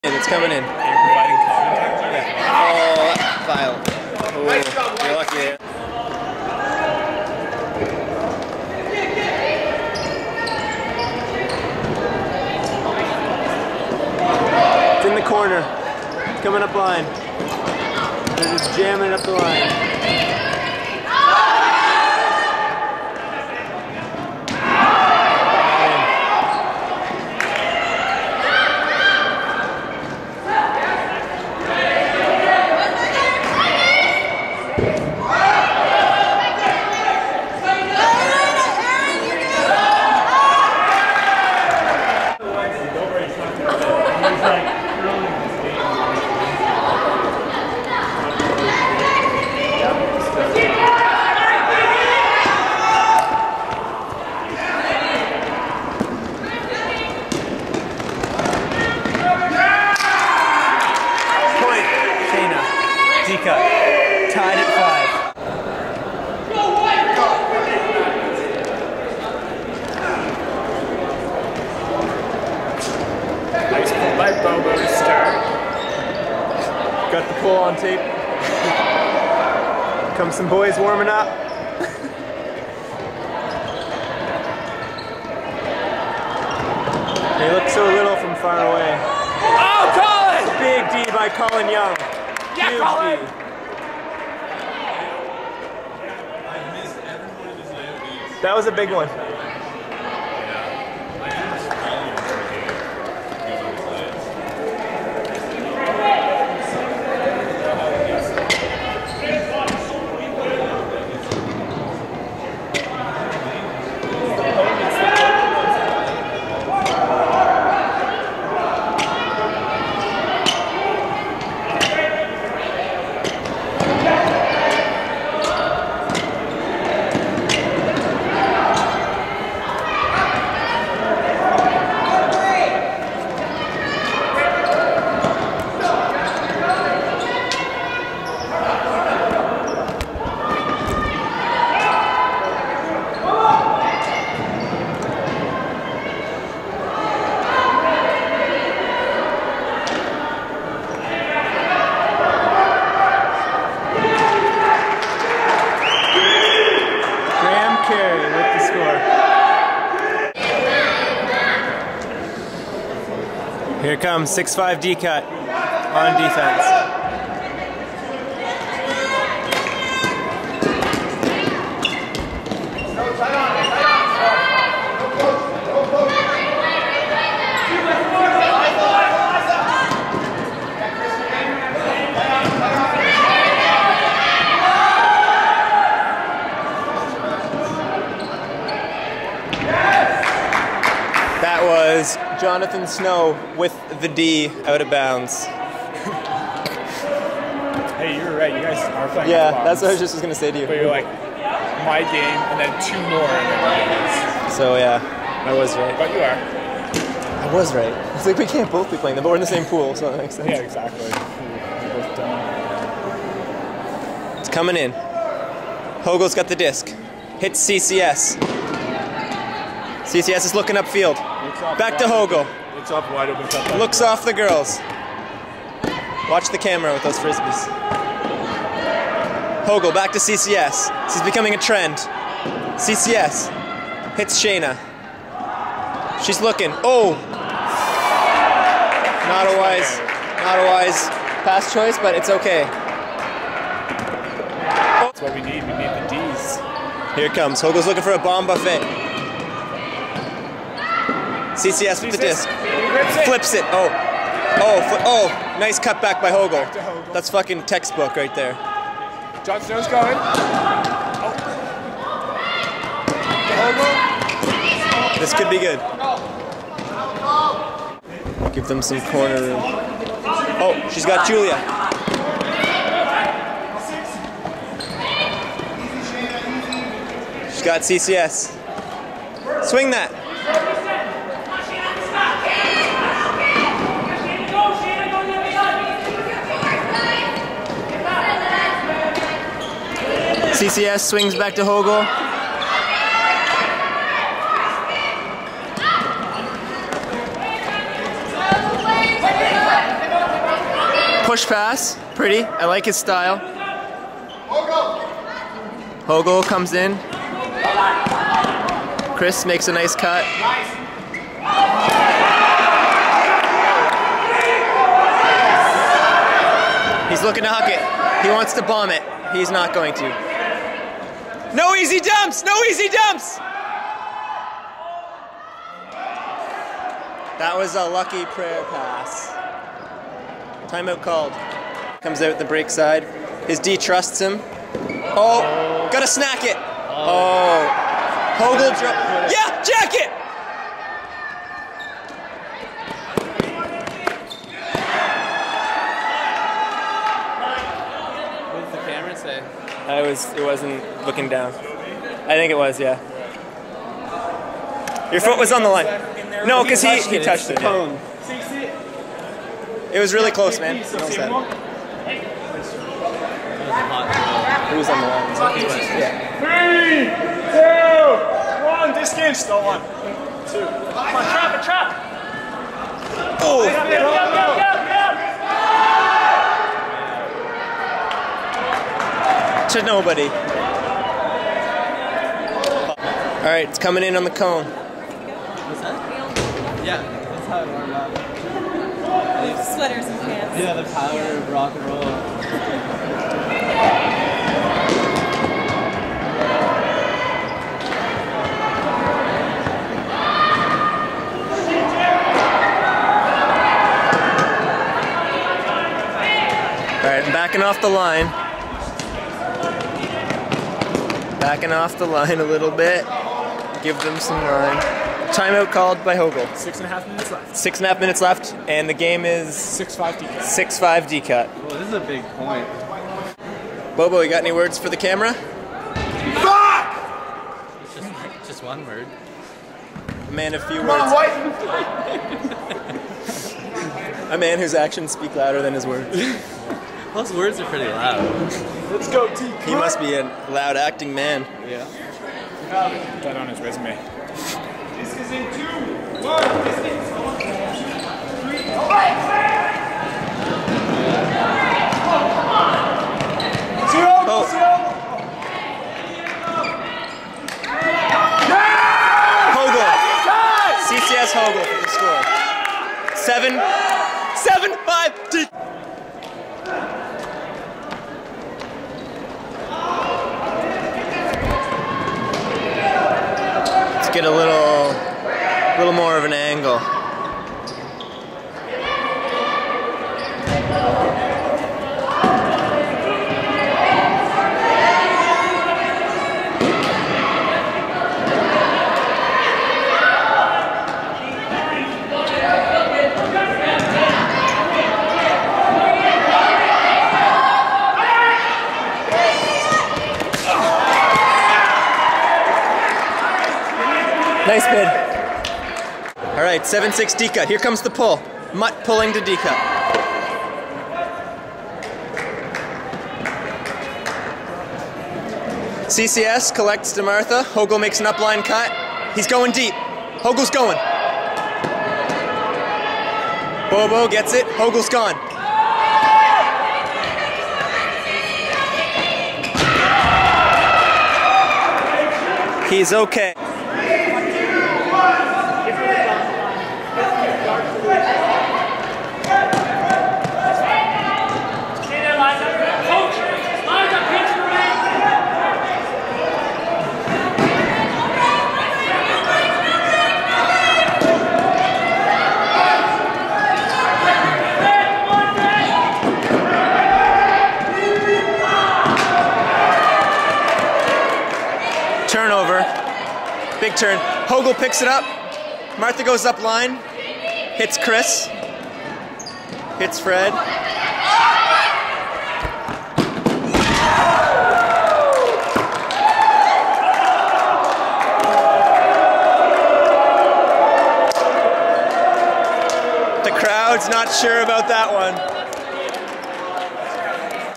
FruitBowl 2010: DKUT vs MUTT Pt II
DKUT (Dalhousie Kings Ultimate) vs MUTT (Moncton Ultimate) at FruitBowl 2010. Unedited game-tape.